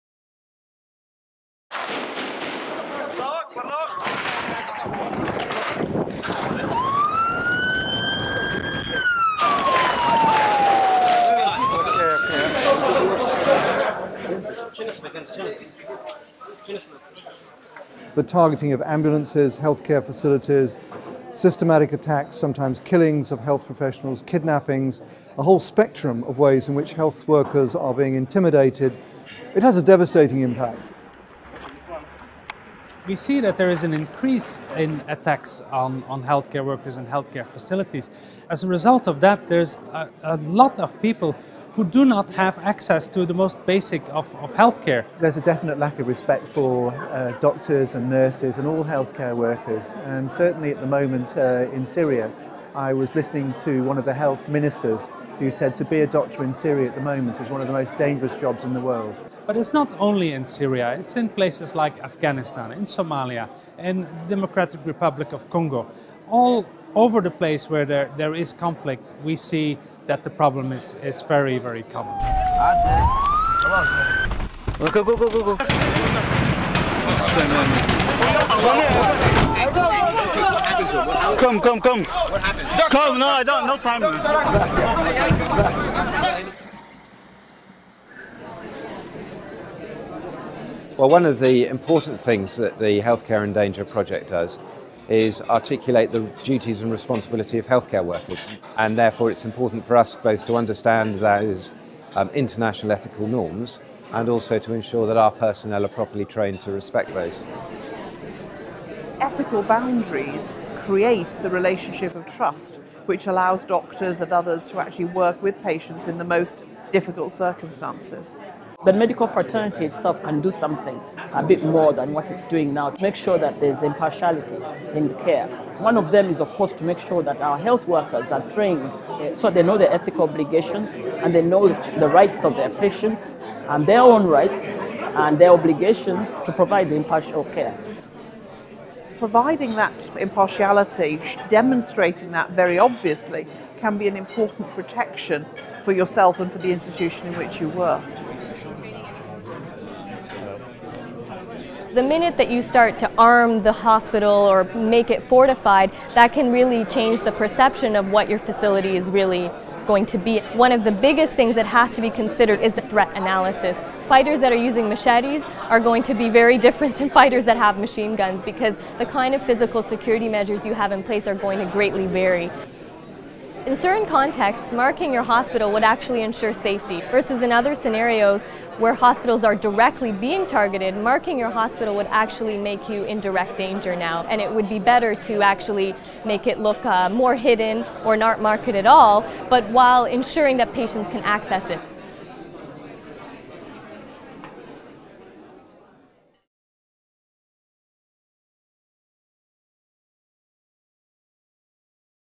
Health Care In Danger: Seeking solutions
Attacks on health services are a growing problem in the world's conflict zones and have a devastating impact on health care. Health experts gathered in London at the Royal Society of Medicine in December 2013 responded with alarm to on doctors in Syria and elsewhere. They discussed solutions, including the responsibility of health workers to treat patients impartially. This film traces the path from problem to solutions and highlights images from the Health Care in Danger campaign.